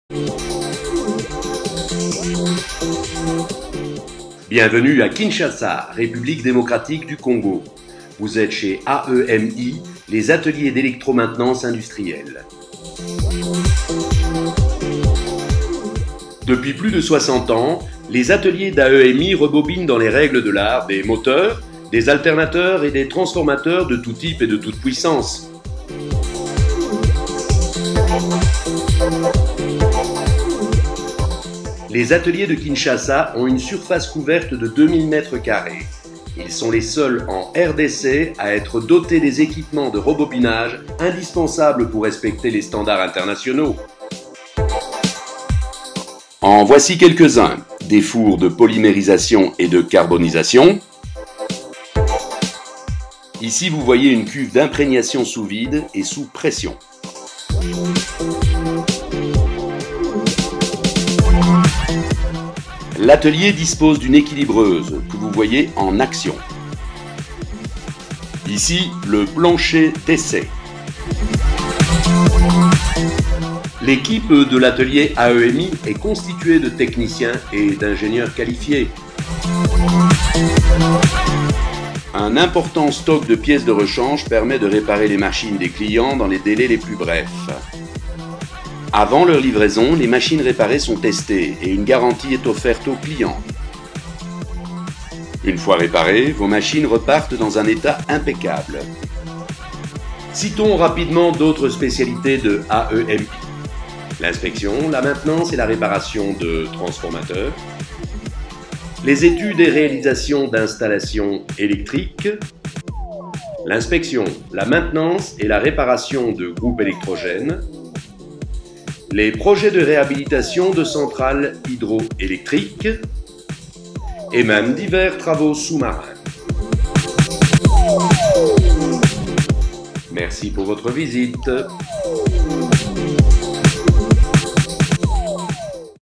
Les ateliers AEMI à Kinshasa
Bienvenue à Kinshasa, République démocratique du Congo. Vous êtes chez AEMI, les ateliers d'électro maintenance industrielle. Depuis plus de 60 ans, les ateliers d’AEMI re-bobinent dans les règles de l’art des moteurs, des alternateurs et des transformateurs de tous types et de toutes puissances. Les ateliers de Kinshasa ont une surface couverte de 2000m2. Ils sont les seuls en RDC à être dotés des équipements de re-bobinage indispensables pour respecter les Standards Internationaux. En voici quelques uns : des fours de polymérisation et de carbonisation.  Ici vous voyez une cuve d’imprégnation sous vide et sous pression. L'atelier dispose d'une équilibreuse que vous voyez en action. voici le plancher d'essai. L'équipe de l'atelier AEMI est constituée de techniciens et d'ingénieurs qualifiés. Un important stock de pièces de rechange permet de réparer les machines des clients dans les délais les plus brefs. Avant leur livraison, les machines réparées sont testées et une garantie est offerte au client. Une fois réparées, vos machines repartent dans un état impeccable. Citons rapidement d'autres spécialités de AEMI: L'inspection, la maintenance et la réparation de transformateurs. Les études et réalisations d'installations électriques. L'inspection, la maintenance et la réparation de  de groupes électrogènes.Les projets de réhabilitation de centrales hydro électriques et même les travaux sous marin.